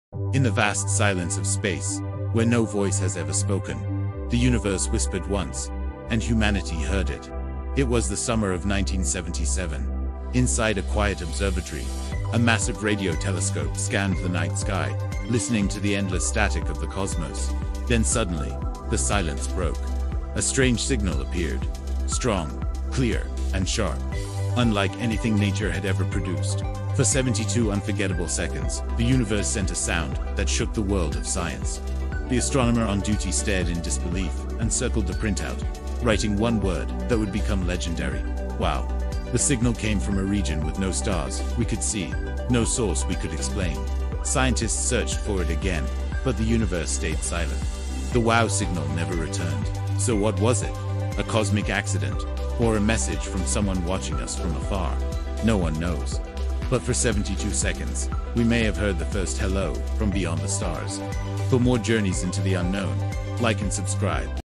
he WOW Signal: The First Mysterious Space Message Humanity Ever Detected!
The Space Signal That Shook Science… The WOW Mystery Explained

72 Seconds That Changed Everything… The Universe Spoke, We Heard

The Most Mysterious Signal from Space: Are We Alone?

📝 YouTube Description (SEO-Optimized & Engaging)

In 1977, something extraordinary happened…
A powerful and unexplained signal from deep space struck a radio telescope in Ohio.
It lasted 72 seconds—and then disappeared forever.

This is the true story of the WOW Signal,
the first mysterious space transmission that scientists still cannot explain.

🌌 In this video, you will discover:
• What the WOW Signal really was
• Why scientists believe it might be intelligent
• Why it appeared once and never again
• Theories about extraterrestrial communication
• Why this 72-second signal remains one of the greatest cosmic mysteries

Was it a message?
A cosmic accident?
Or proof that we are not alone?

Join us as we dive into one of the strangest and most fascinating events in the history of space exploration.